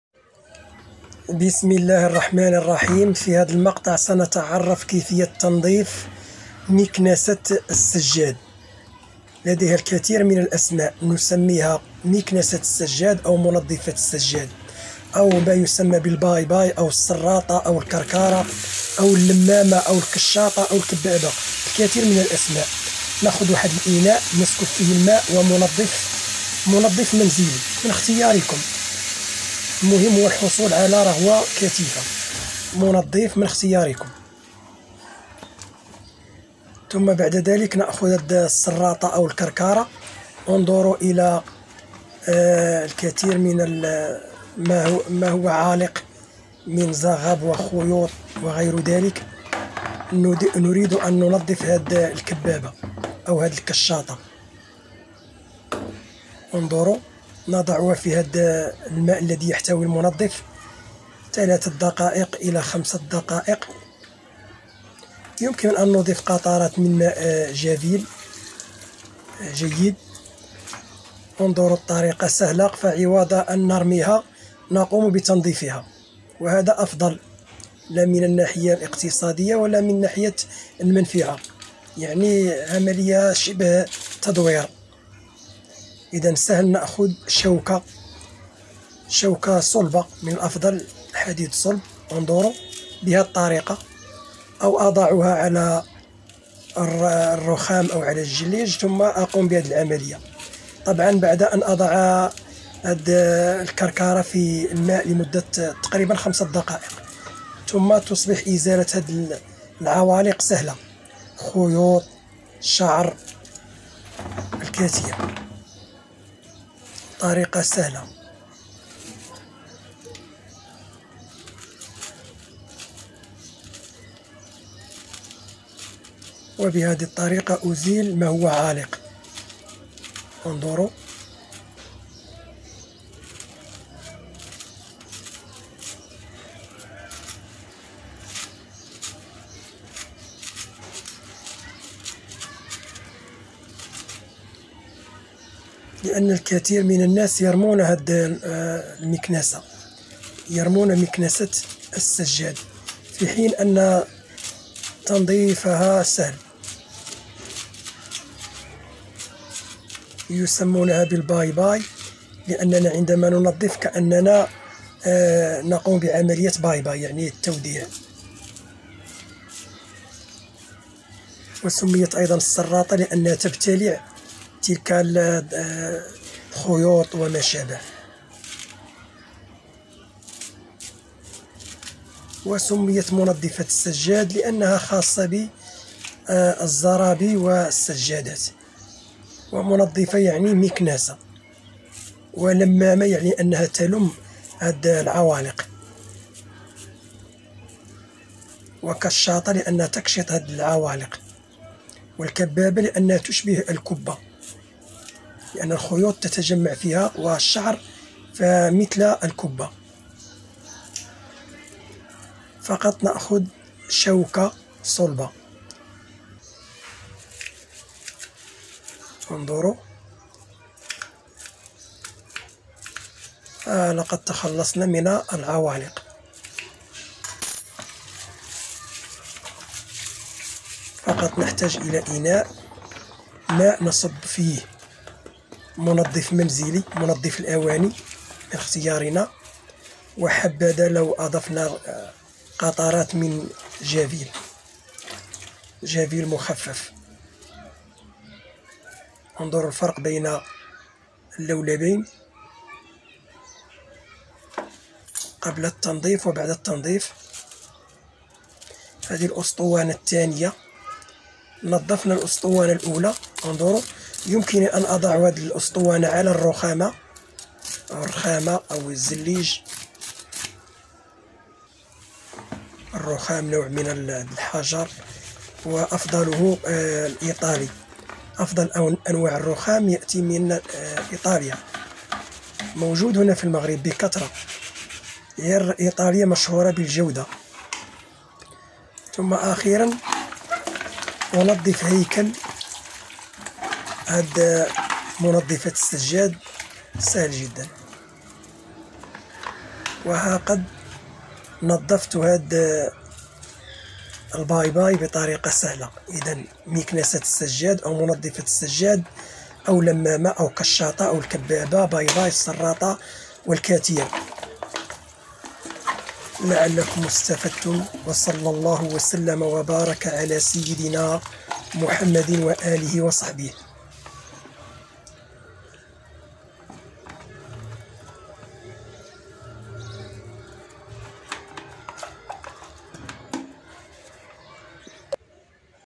طريقة تنظيف الكركارة،مكنسةالزربية،المشاطة ،الكشاطة،الكبابة،لمامة،باي باي ،السراطة
تنظيف مكنسة الزرابي والسجاد بطريقة سهلة اقتصادية